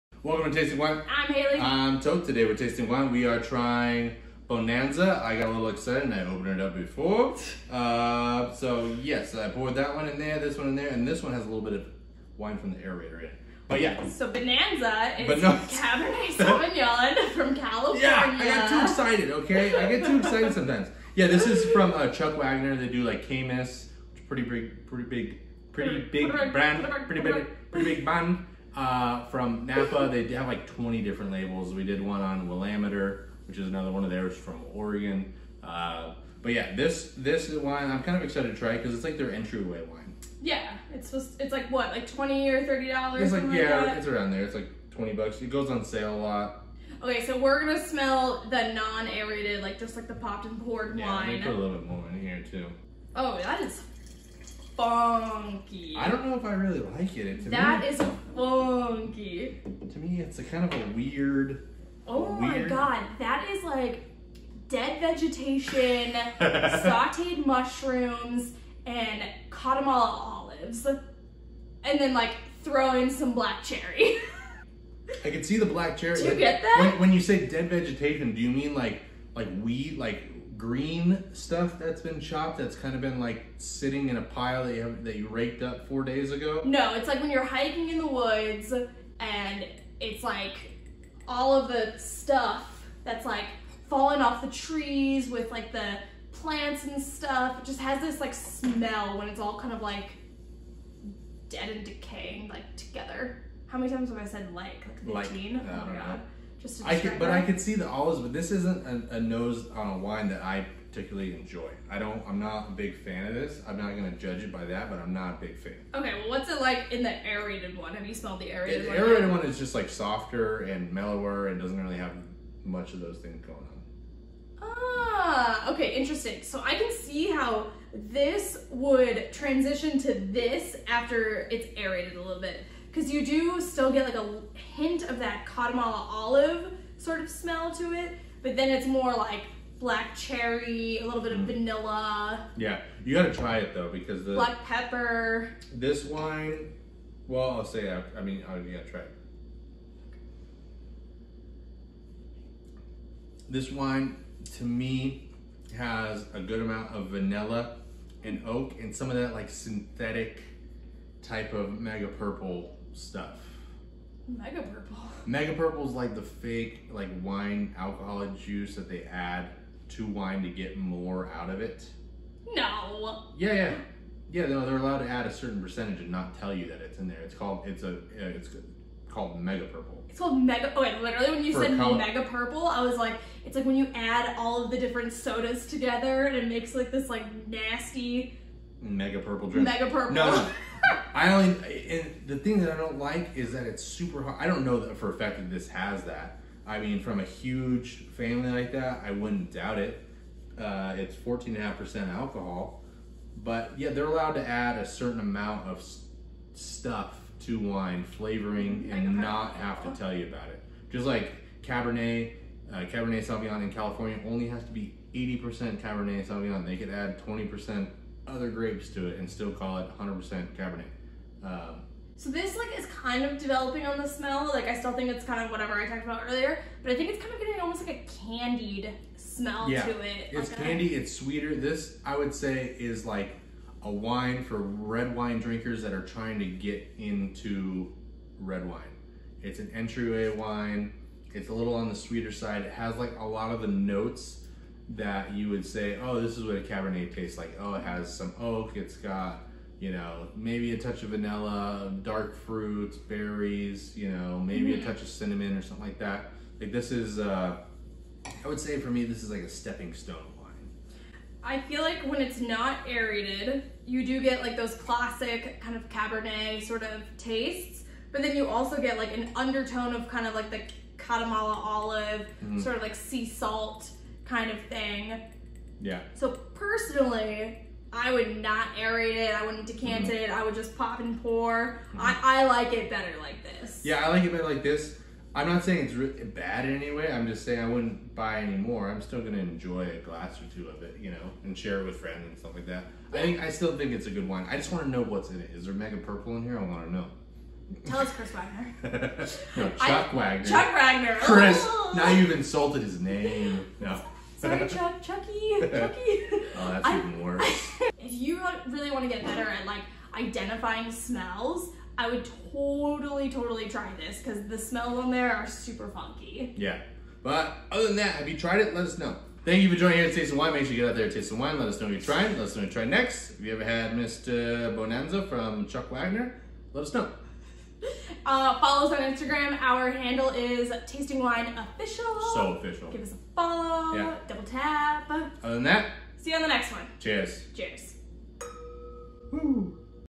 Bonanza Cabernet Sauvignon Wine Review
Today were are tasting Bonanza Cabernet Sauvignon. Follow us along on our wine tasting journey! Hayley and Toth are not experts, we're here to taste different wines and learn more!
If you have a wine you would like us to review leave it in the comments below.

Zalto Champagne Glasses
Glass Wine Decanter